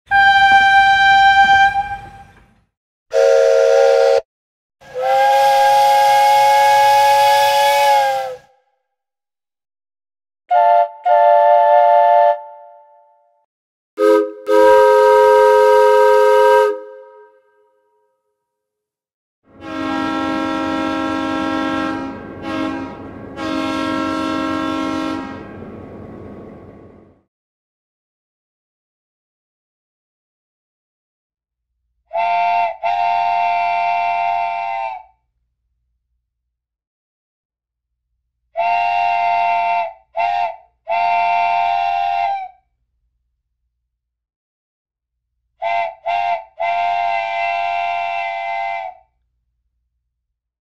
Train Whistle Sound Effect 🚂📢🔊
In this video, we bring you the most realistic train whistle sound effects for your videos. Whether you're creating a short film, a documentary, or a commercial, adding a train whistle sound can add a sense of nostalgia and adventure to your content. Our collection includes a variety of train whistle sounds, from distant and haunting to loud and powerful, all recorded in high-quality audio. We also provide tips on how to use these sound effects effectively in your videos, and how to mix them with other audio elements. So, if you want to take your videos to the next level and transport your audience to a different time and place, check out our train whistle SFX collection today!